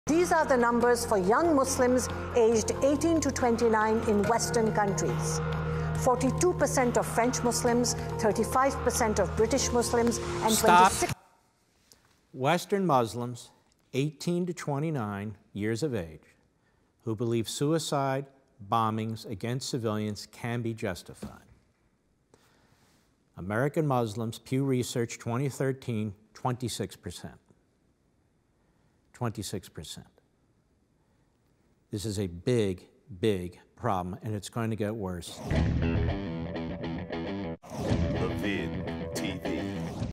LevinTV: A Big Problem
According to a 2013 Pew Research study, 26% of American Muslims believe suicide bombings against civilians can be justified. This is a big problem.